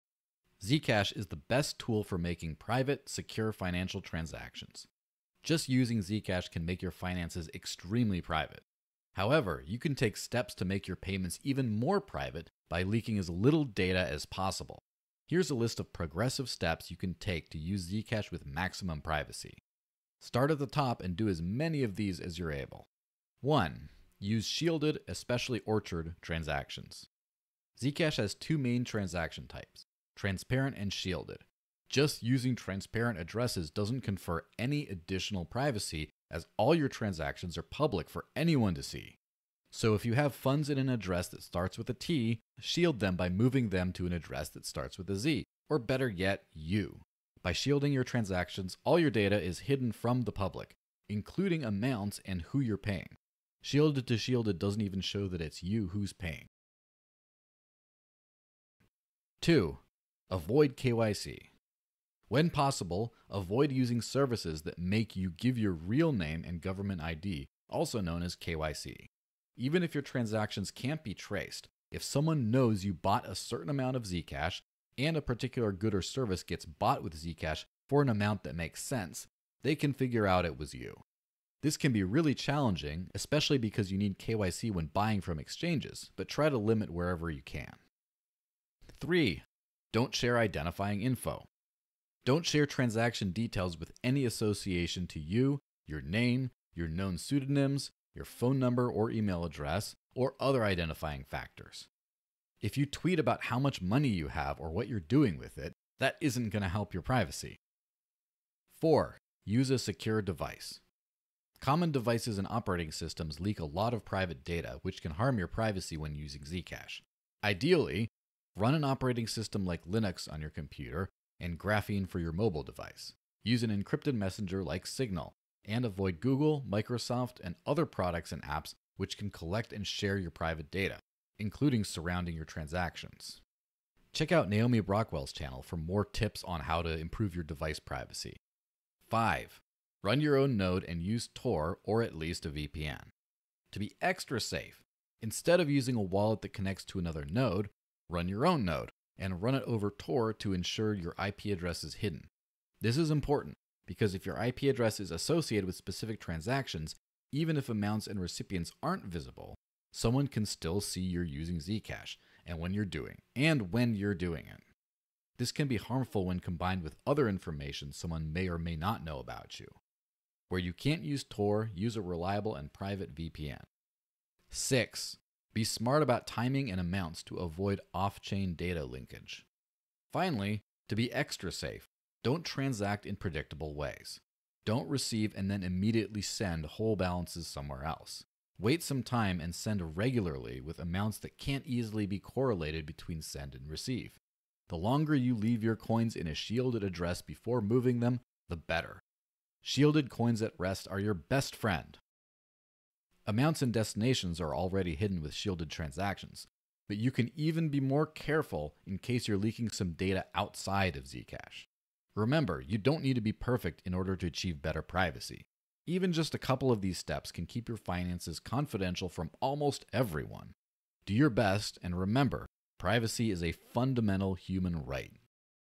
Useful Tips when using Zcash 🔒
Learn important methods of keeping your financial information secure when using Zcash.

Best Practices 💁‍♀️
- Use Shielded Transactions
- Use a Secure Device
- Run your own Node
- Don't share identifying Info